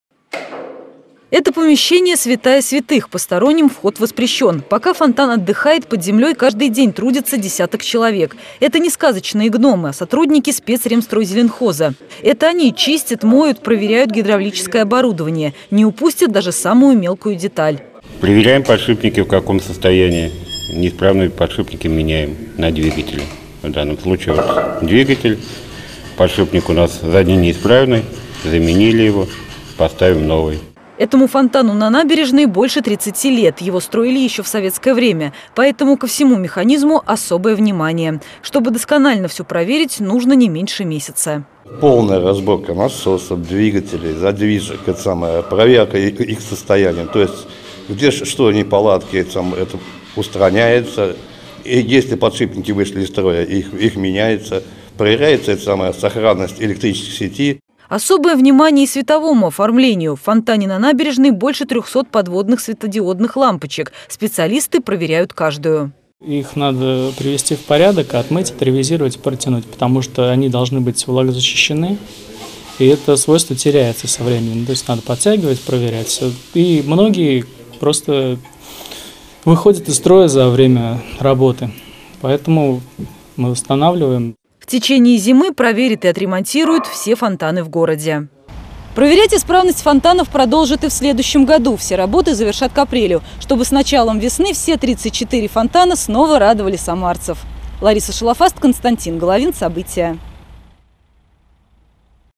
Самарский фонтан на набережной "прошел" тщательный профосмотр
Пока одни готовятся к празднику, другие думают о  весне. Самарские фонтаны - а в городе их 34 -только внешне впали в спячку.  На самом же деле  им устроили  тщательный профосмотр.  Многим сооружениям уже не один десяток лет, поэтому внимание - к каждой детали. Все работы идут под землей.